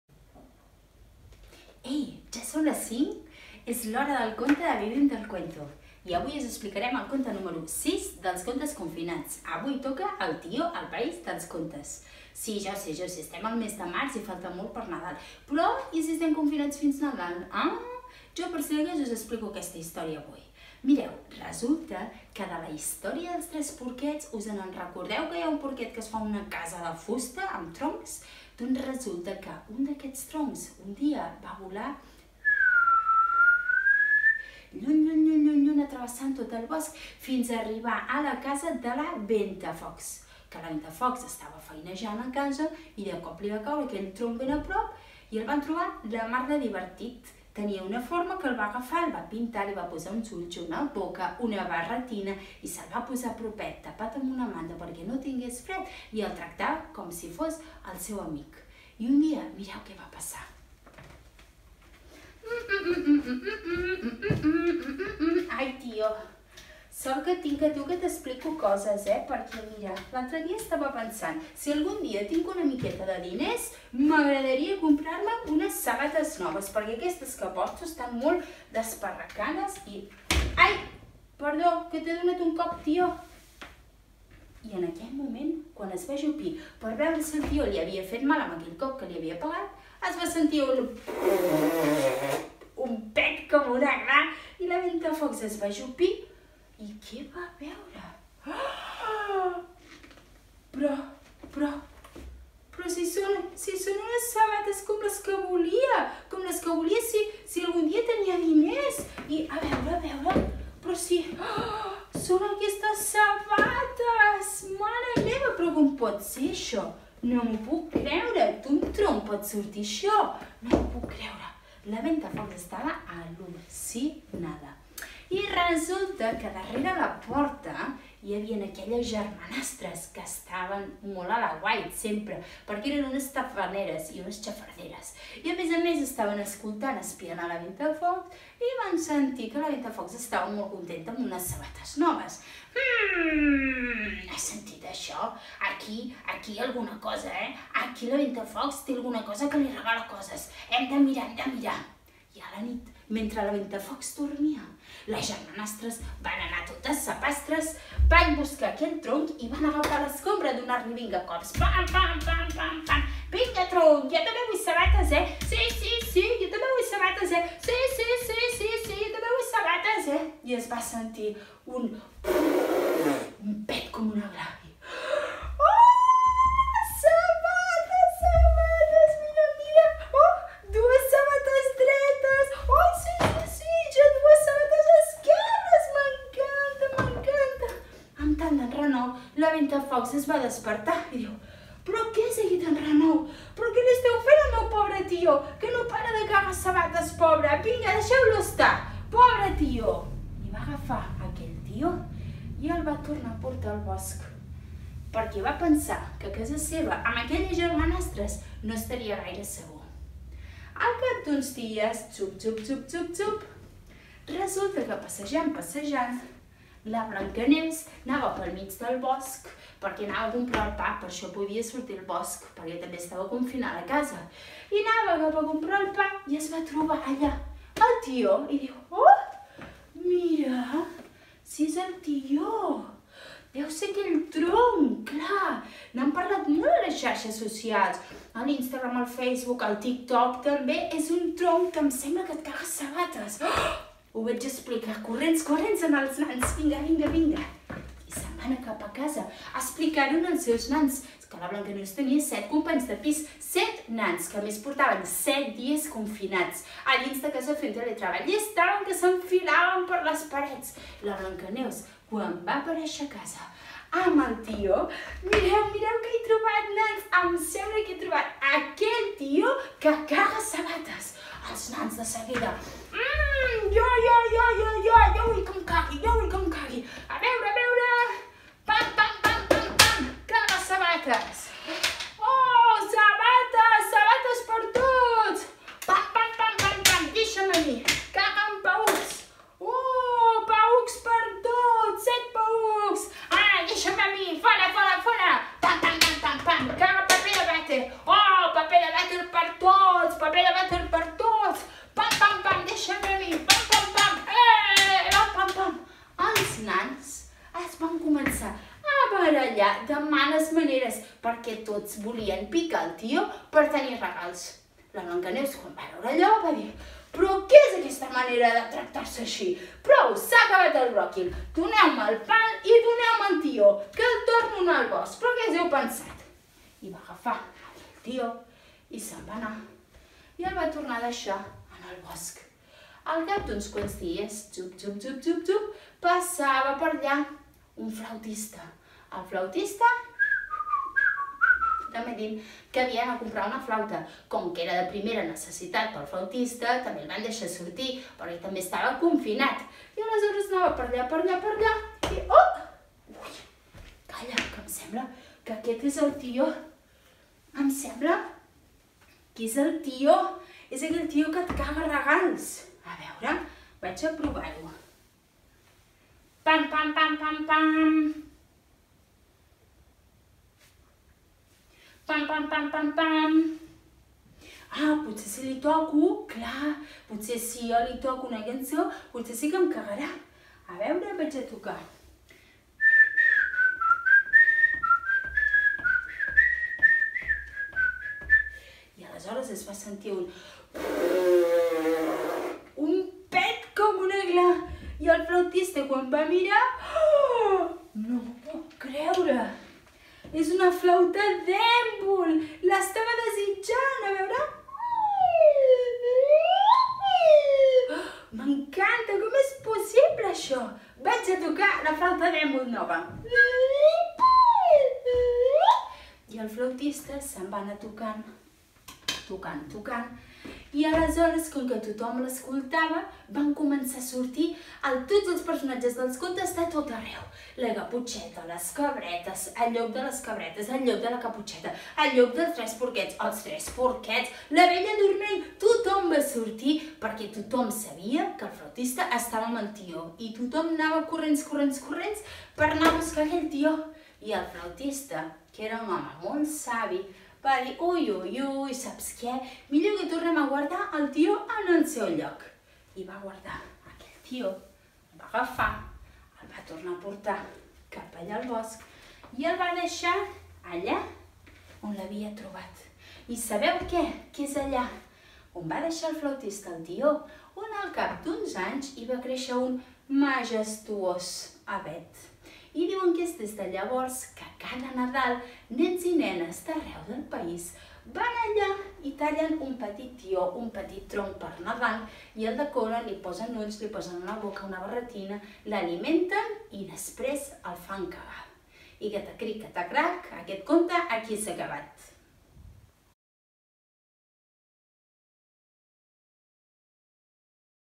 El tió al país dels contes (Contes confinats 6)
El grup Vivim del Cuentu, 8 rondallaires que fan arribar els contes i les històries de la tradició a petits i grans d’arreu del país, avui ens explica la història d'un tió molt especial!

T'esperem demà amb "Els quatre porquets"